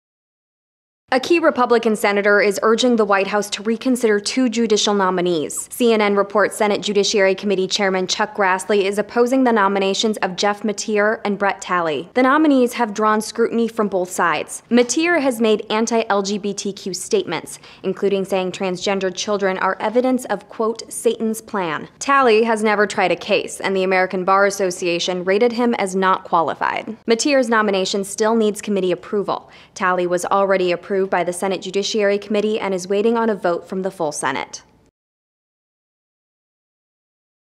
GOP's Grassley against 2 Trump judicial picks
CNN reports Senate Judiciary Committee Chairman Chuck Grassley urged the White House to reconsider nominees Jeff Mateer and Brett Talley.